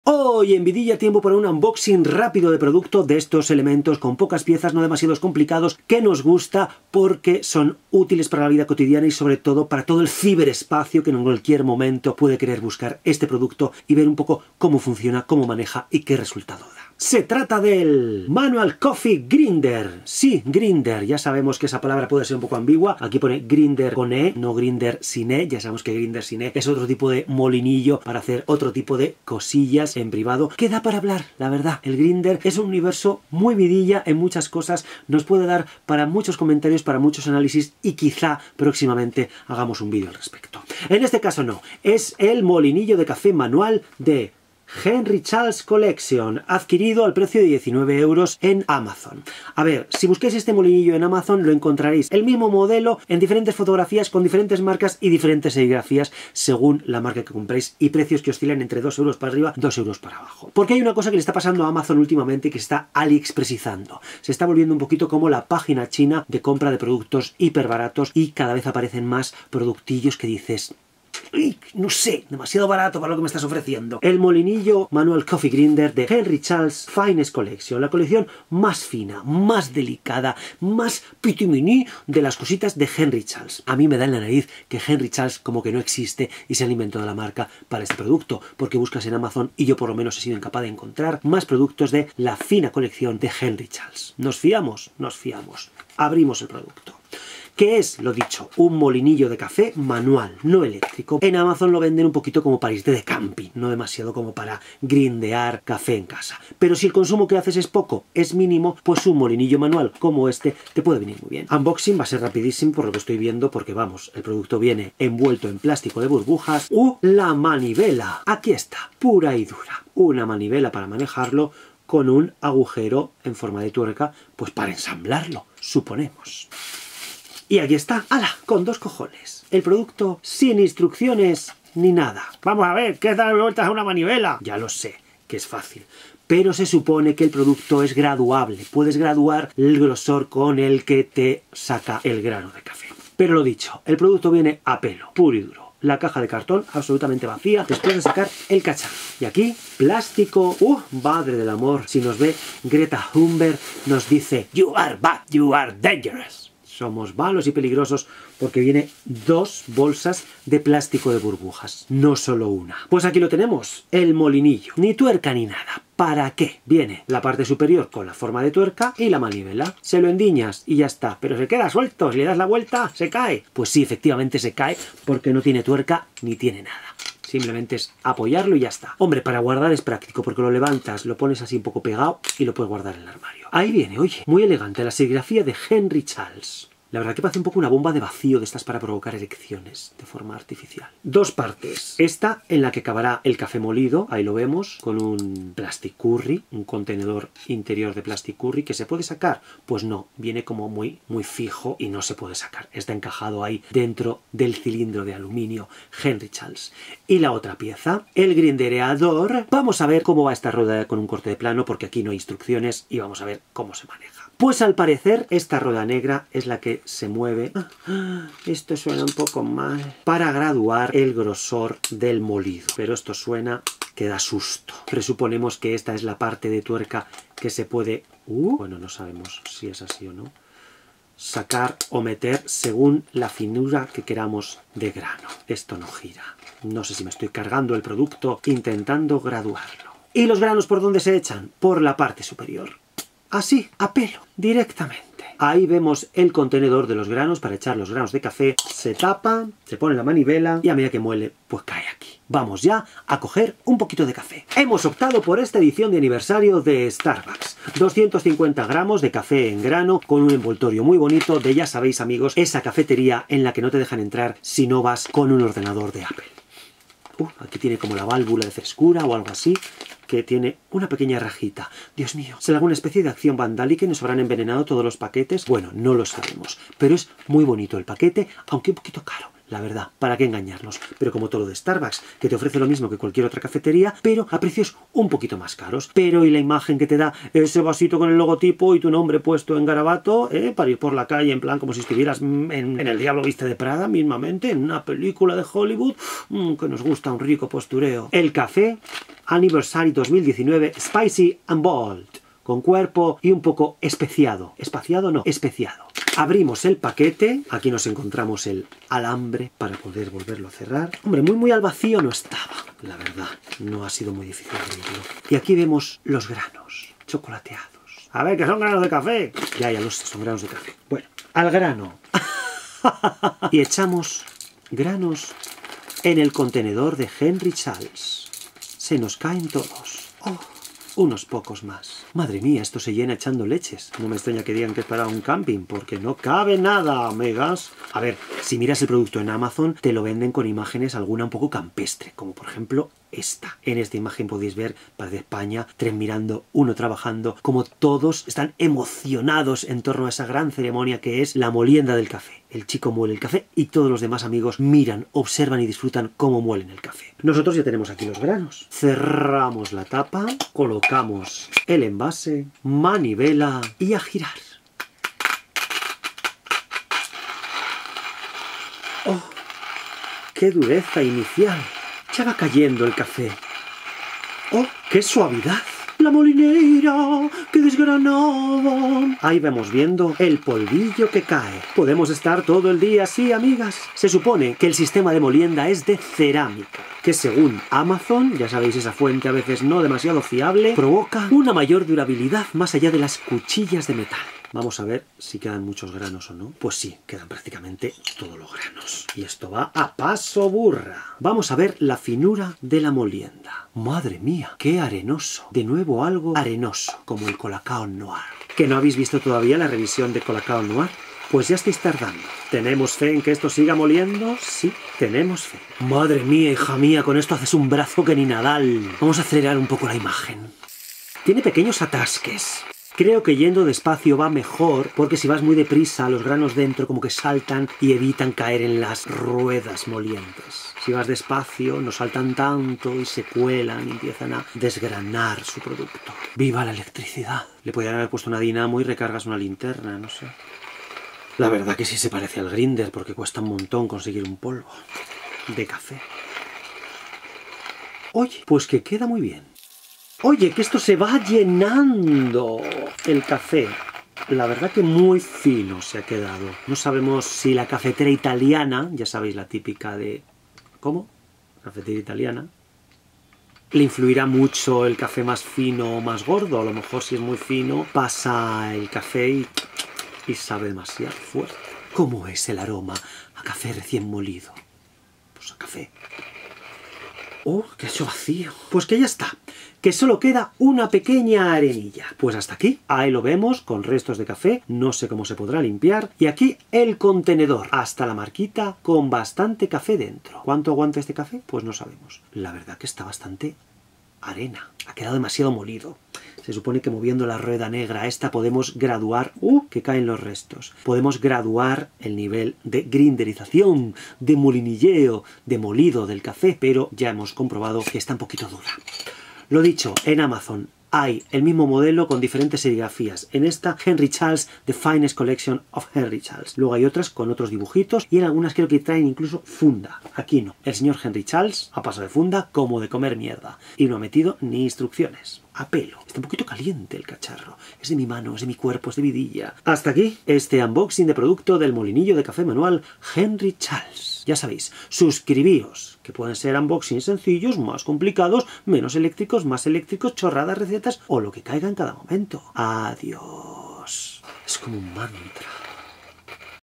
UNBOXING - Molinillo manual de café Henry Charles
Vidilla se pone cafetero y nos enseña en unboxing un molinillo manual de café.